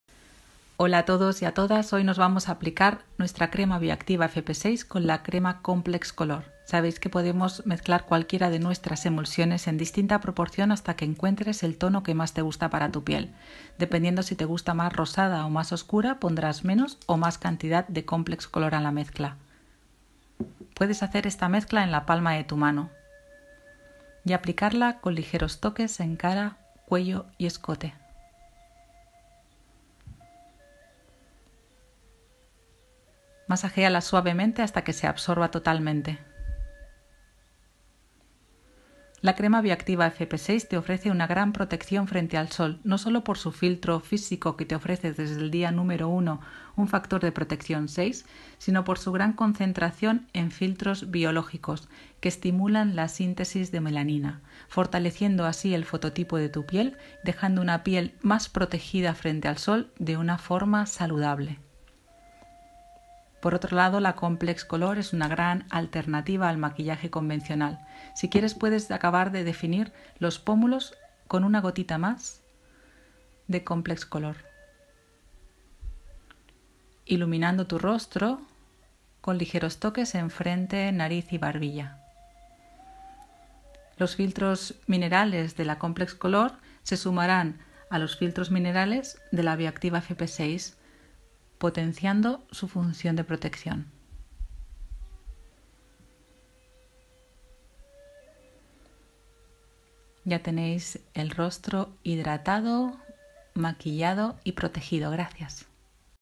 PASO A PASO Protección solar efecto buena cara Crema bioactiva FP6 y Crema complex color
Cómo mezclar la Crema complex color con la Crema bioactiva FP6

¿quieres salir todas las mañanas de casa con buena cara?
Hoy os mostramos cómo puedes hacerlo de una forma natural, hidratando, nutriendo, reafirmando y protegiendo tu piel del sol, aportando un bronceado natural a tu piel.
Embellece tu piel.
Además de aplicarte energía correctora y regeneradora, con protección solar, vas a embellecer sin incluir ni asfixiar tu piel.
Armoniza la epidermis, unificando y realzando tu tono, matizando y tratando manchas.
Contiene pigmentos de arcillas que depuran, absorben impurezas, calman, desestresan y desintoxican tu piel.
Mostramos todos los pasos de esta exquisita mezcla para lucir una piel radiante y luminosa, y así, sacar el máximo partido a nuestros biocosmeticos .